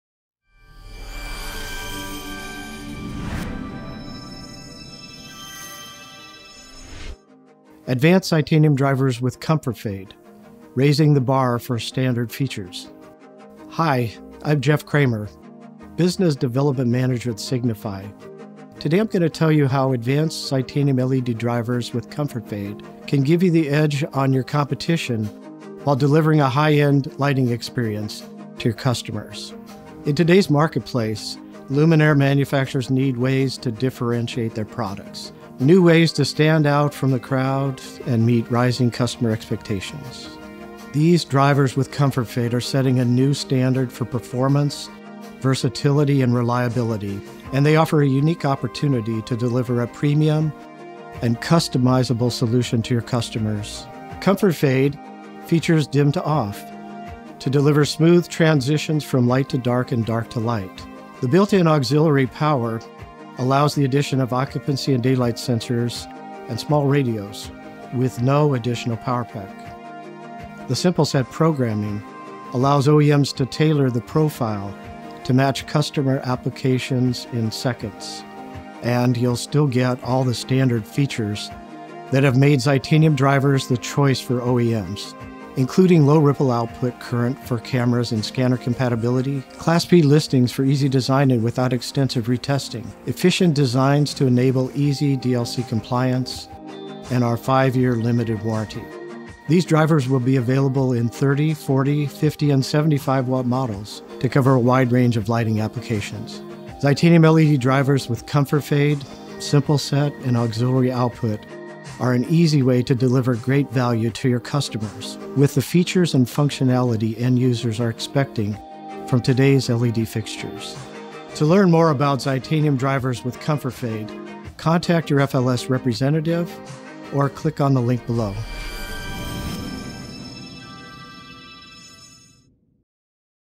Signify Advance Xitanium Drivers With Comfortfade: Raising The Bar For Standard Features
Today we are going to show you how Advance Xitanium LED drivers with ComfortFade can give you the edge on your competition while delivering a high-end lighting experience to your customers.

In today’s marketplace, luminaire manufacturers need ways to differentiate their products — new ways to stand out from the crowd and meet rising customer expectations.

These drivers with ComfortFade are setting a new standard for performance, versatility, and reliability, and they offer a unique opportunity to deliver a premium and customizable solution to your customers.

ComfortFade features dim to off, to deliver smooth transitions from light to dark, and dark to light. The built-in auxiliary power allows the addition of occupancy and daylight sensors and small radios with no additional power pack. The SimpleSet programming allows OEMs to tailor the profile to match customer applications in seconds.

And, you’ll still get all the standard features that have made Xitanium drivers the choice for OEMs, including Low-ripple output current for camera and scanner compatibility, Class P listings for easy design-in without extensive retesting, efficient designs to enable easy DLC compliance, and our 5-year limited warranty.

These drivers will be available in 30, 40, 50 and 75 watt models to cover a wide range of lighting applications.

Xitanium LED drivers with ComfortFade, SimpleSet, and auxiliary output are an easy way to deliver great value to your customers, with the features and functionality end users are expecting from today’s LED fixtures.

To learn more about Xitanium drivers with ComfortFade, contact your FLS representative.